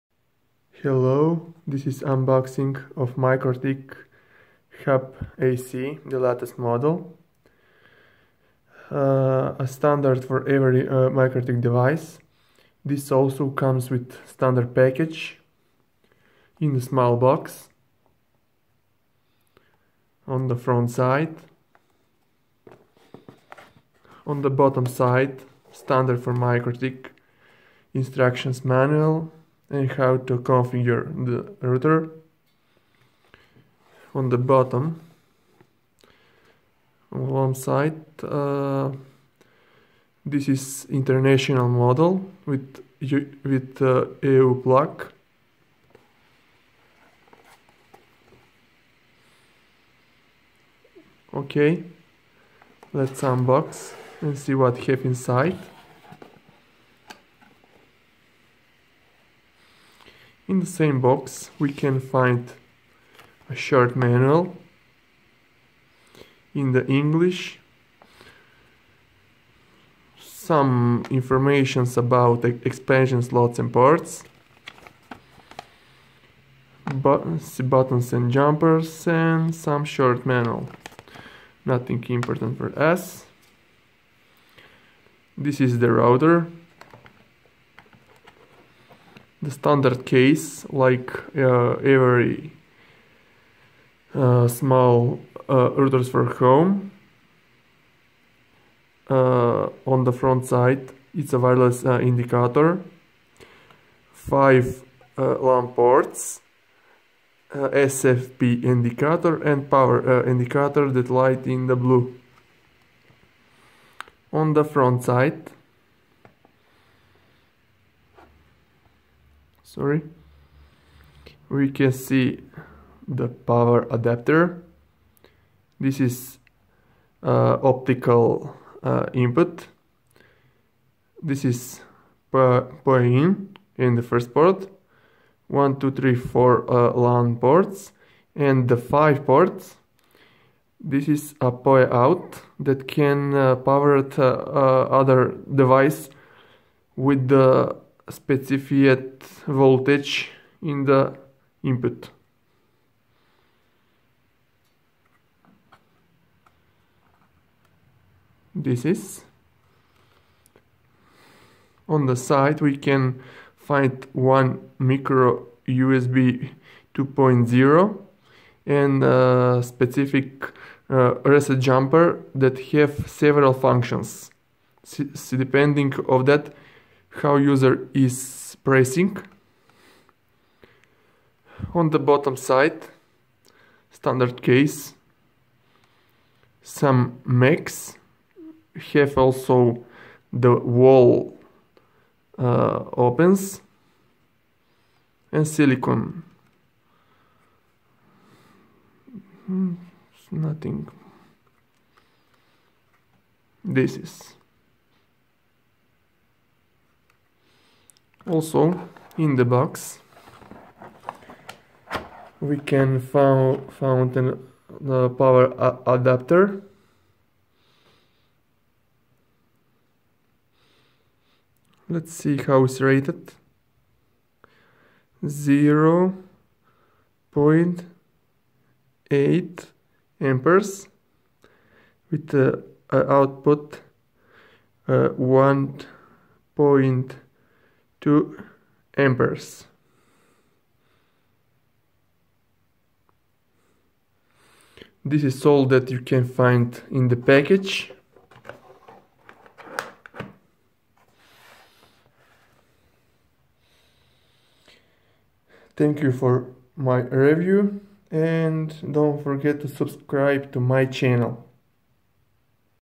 Mikrotik HAP AC Unboxing and First Look
This is unboxing and first look of my Mikrotik RouterBoard hAP AC that i purchased recently.  Product URL